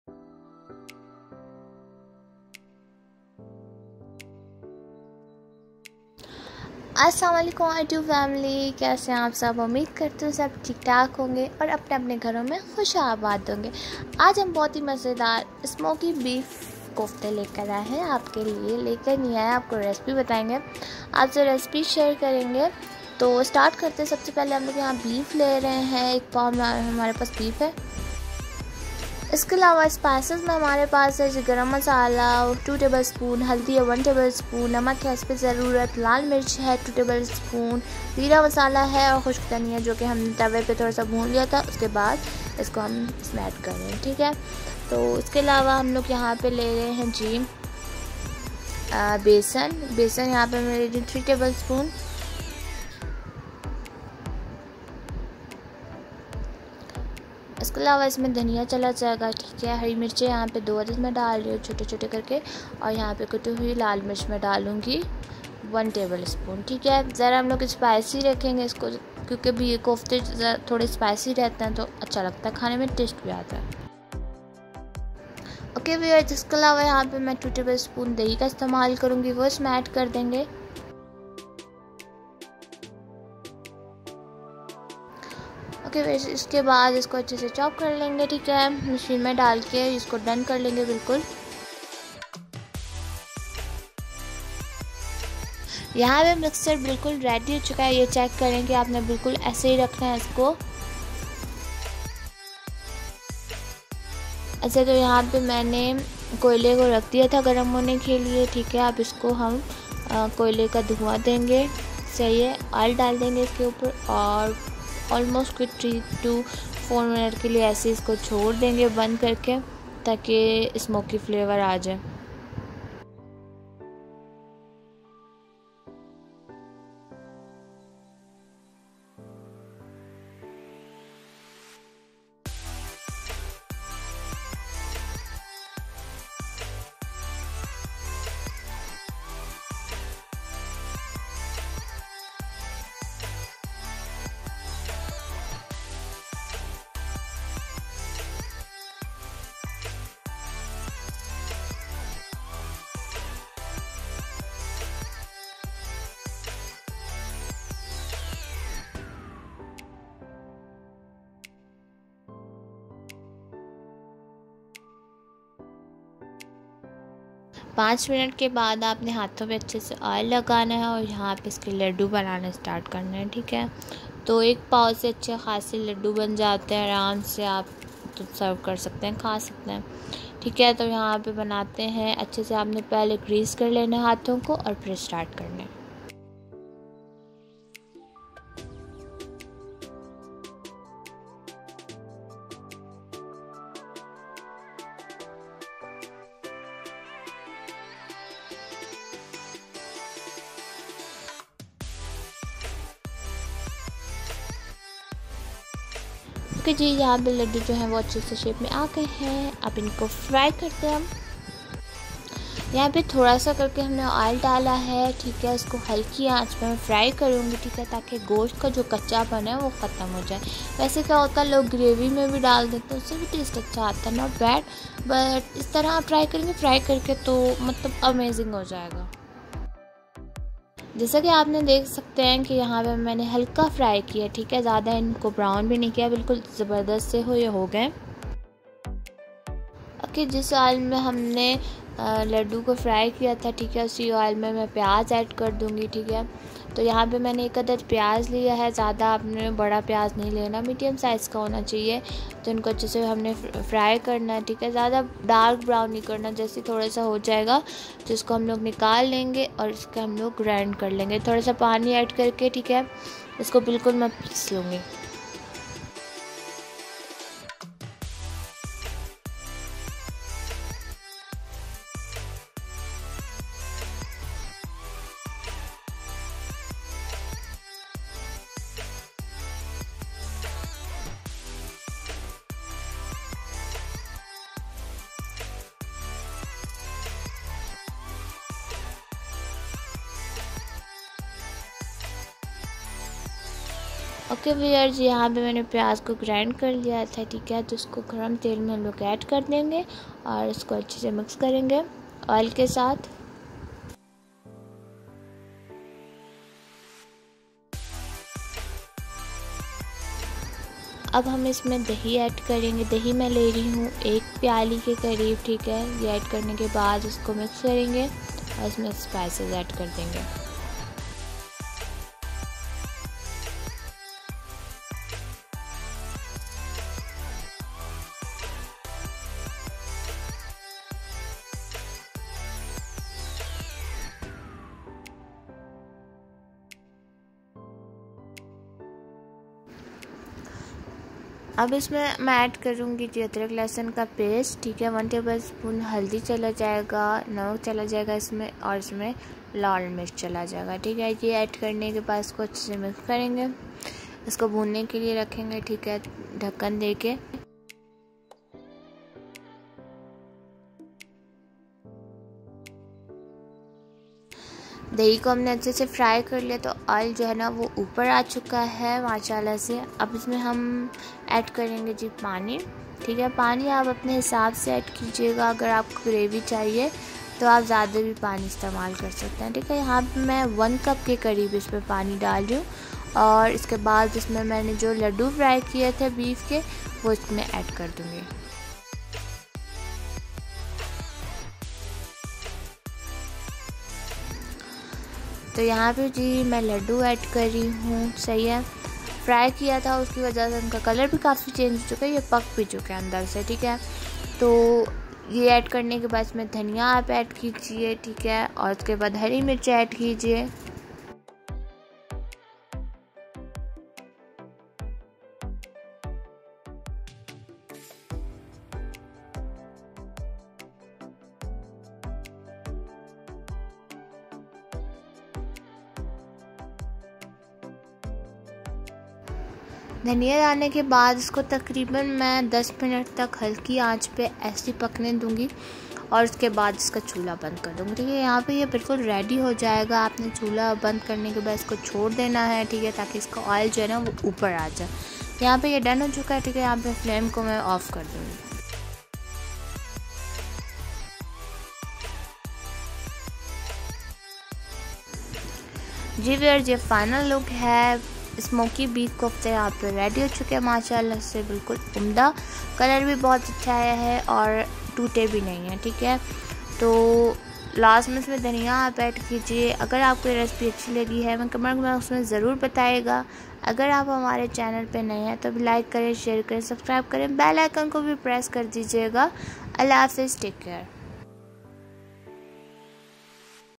Smoky Beef Kofta Recipe | Kofta Curry | Meat Balls Recipe | Koftay Ka Salan | Girls Healthy Journey
Recipe : Half cup vegetable oil
1yellow onion sliced
1 teaspoon ginger garlic paste
1 cup yogurt
Half Teaspoon turmeric powder                                                                                   1 Tablespoon red chili powder
As you like Tablespoon salt                                                                                                   FOR MEATBALLS
250 g beef mince
3 tablespoon yogurt
1 teaspoon garam masala
2 teaspoon red chili powder
1 teaspoon coriander powder Roasted
1 teaspoon cumin powder Roasted
1 teaspoon salt
3 tablespoon gram flour roasted
2 green chilies chopped
2 tablespoon coriander chopped                                                                                 2 tablespoon Yogurt